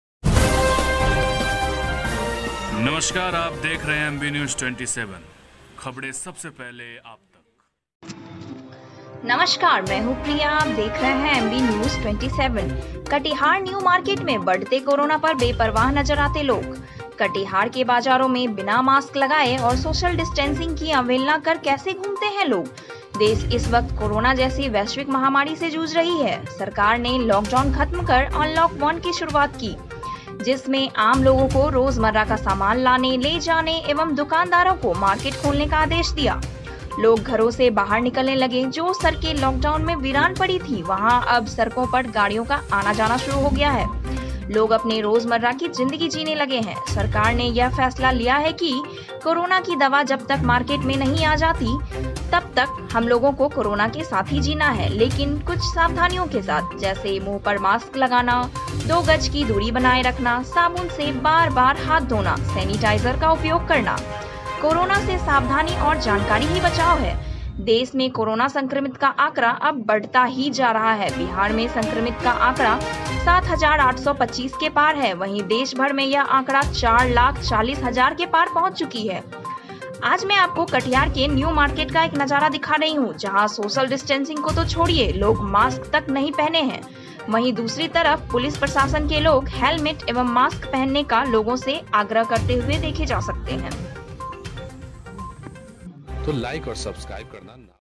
(कटिहार न्यू मार्केट में बढ़ते कोरोना पर बेपरवाह नजर आते लोग)।
कटिहार के बाजारों में बिना मास्क लगाए और सोशल डिस्टेंसिंग की अवहेलना कर कैसे घूमते हैं लोग। देश इस वक्त कोरोना जैसी वैश्विक बीमारी से जूझ रही है। सरकार ने लॉकडाउन खत्म कर अनलॉक वन की शुरुआत की जिसमें आम लोगों को रोजमर्रा का सामान लाने, ले जाने एवं दुकानदारों को मार्केट खोलने का आदेश दिया। लोग घरों से बाहर निकलने लगे जो सरके लॉकडाउन में वीरान पड़ी थी वहां अब सड़कों पर गाड़ियों का आना-जाना शुरू हो गया। लोग अपने रोजमर्रा की जिंदगी जीने लगे हैं। सरकार ने यह फैसला लिया है कि कोरोना की दवा जब तक मार्केट में नहीं आ जाती तब तक हम लोगों को कोरोना के साथ ही जीना है लेकिन कुछ सावधानियों के साथ जैसे- मुंह पर मास्क लगाना ,2 गज की दूरी बनाए रखना ,साबुन से बार-बार हाथ धोना, सेनिटाइजर का उपयोग करना । कोरोना से सावधानी और जानकारी ही बचाव है। देश में कोरोना संक्रमित का आंकड़ा अब बढ़ता ही जा रहा है। बिहार में संक्रमित का आंकड़ा 7825 के पार है। वही देश भर में यह आंकड़ा 4 लाख 40 हजार के पार पहुंच चुकी है। आज मैं आपको कटिहार के न्यू मार्केट का एक नजारा दिखा रहा हूं जहां सोशल डिस्टेंसिंग को तो छोड़िए ,लोग मास्क तक नहीं पहने हैं ।वहीं दूसरी तरफ पुलिस प्रशासन के लोग हेलमेट एवं मास्क पहनने का लोगों से आग्रह करते हुए देखे जा सकते हैं